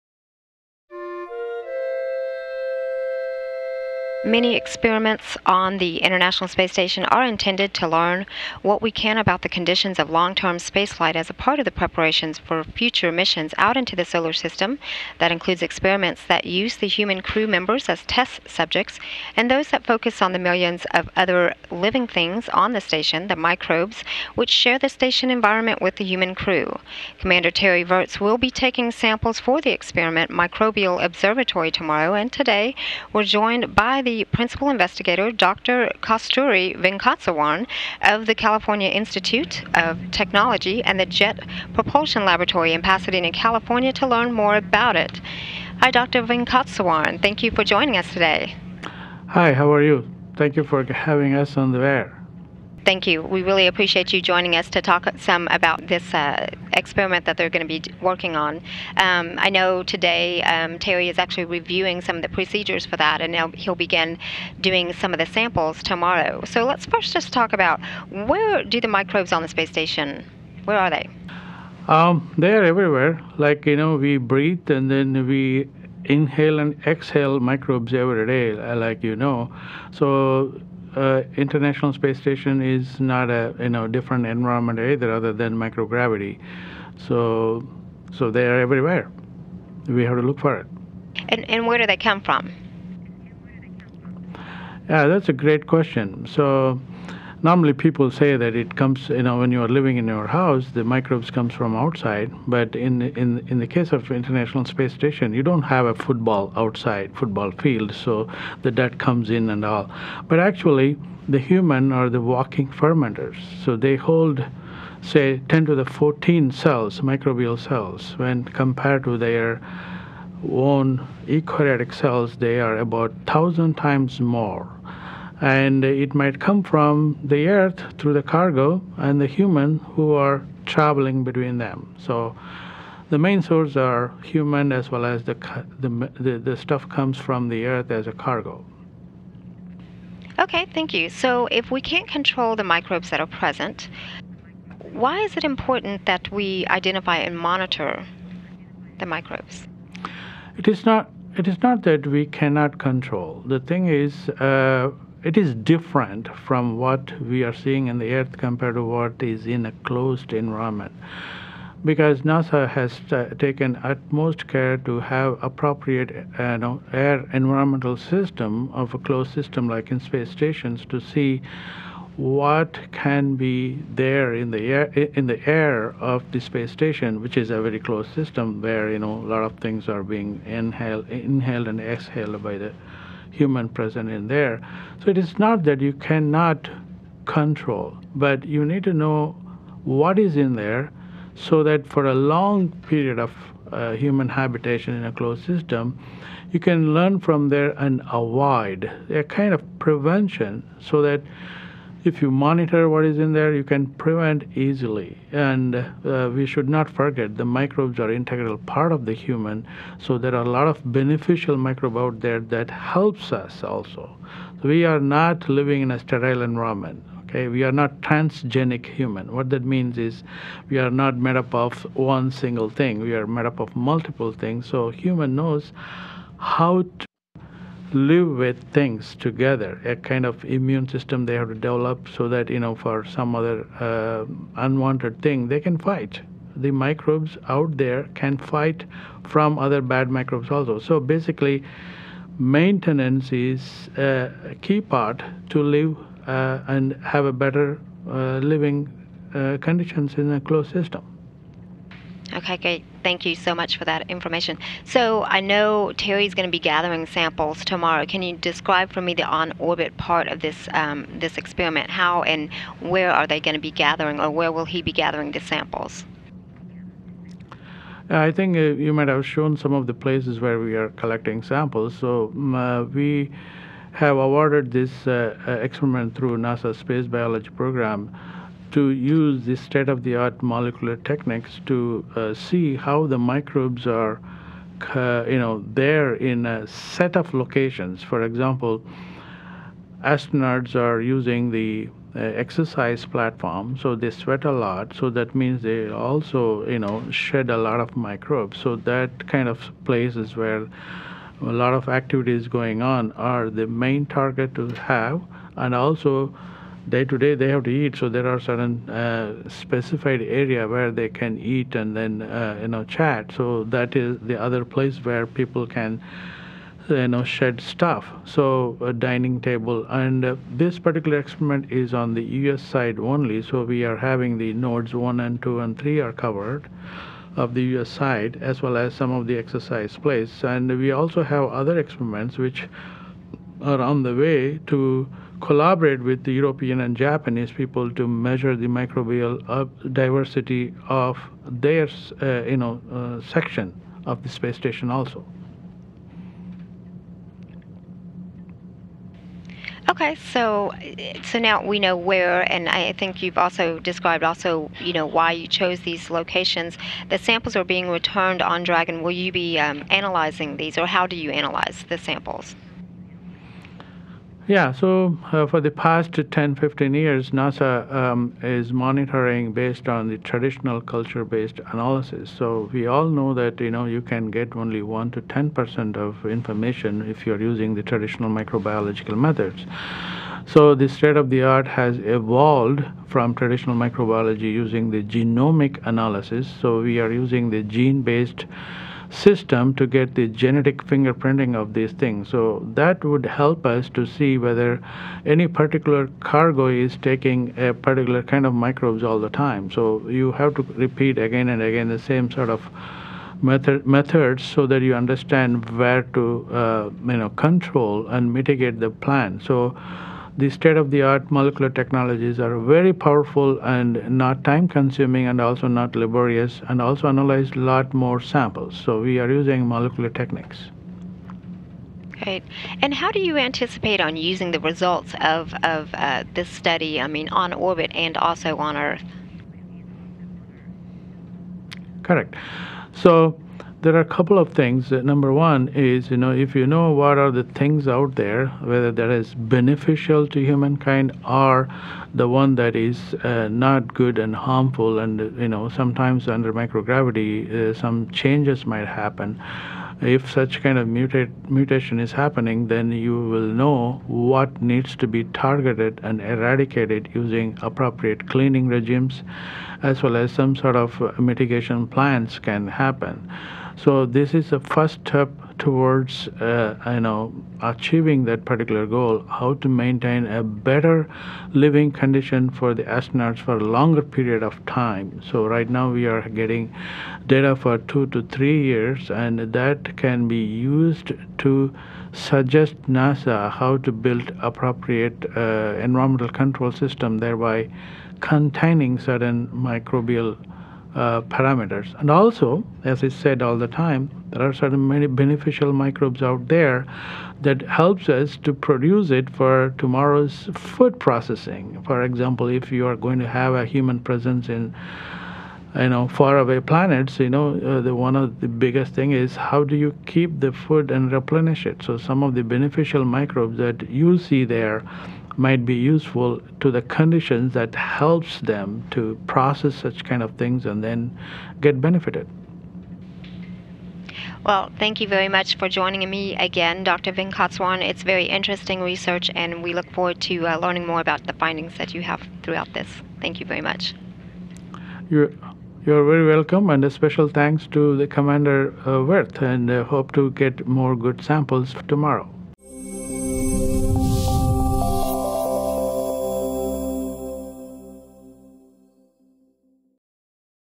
Space Station Live: Millions of microbes under study
NASA commentator Amiko Kauderer talks with Dr. Kasthuri Venkateswaran, principal investigator for the Microbial Observatory experiment aboard the International Space Station that focuses on the millions of other living things on the station other than the crew: the microbes which share the environment with the human crew. Dr. Venkateswaran is with the California Institute of Technology and the Jet Propulsion Laboratory in Pasadena.